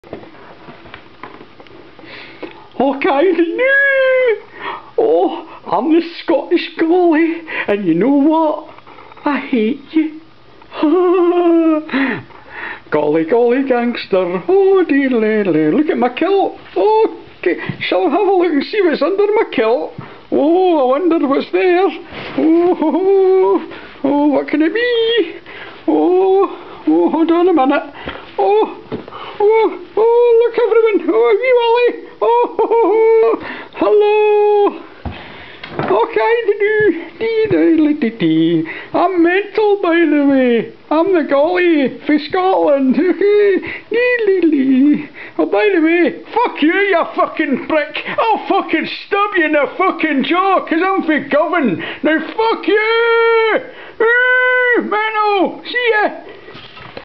scottish golliwog goes nuts
crazy junky kilt wearing gollywog goes all nuts then turns rather dark